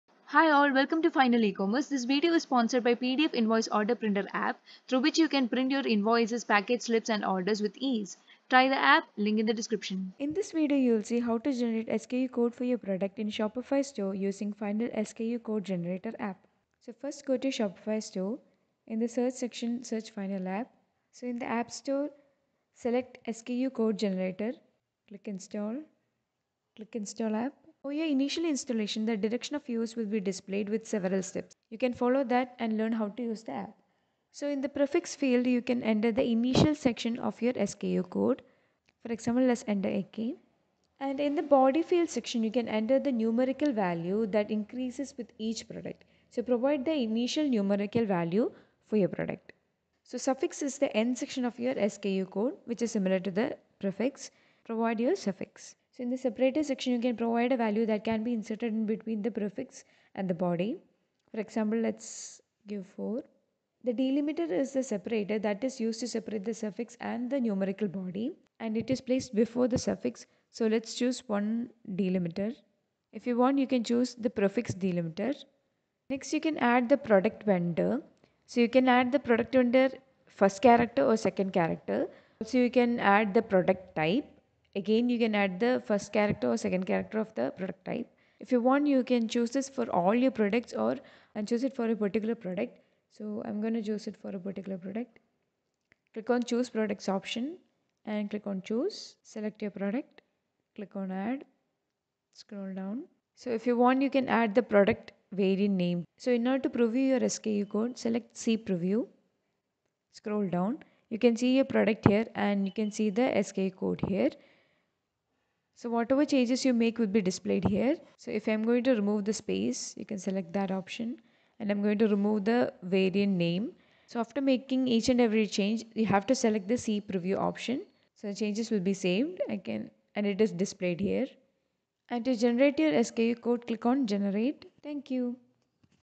How to generate SKU code for your products in Shopify store using Final: SKU Code Generator app.
Generating SKU code for each product is a tedious process. We made it simpler by automatically bulk updating SKU codes based on rules you set. Include prefix & suffix to generated SKUs for any products easily. SKU generator that includes product type and vendor details on SKU.

Easy SKU generator code update in a click of a button.

SKU rules based on your needs for all products.

Bulk edit SKU codes for SKU products in one go.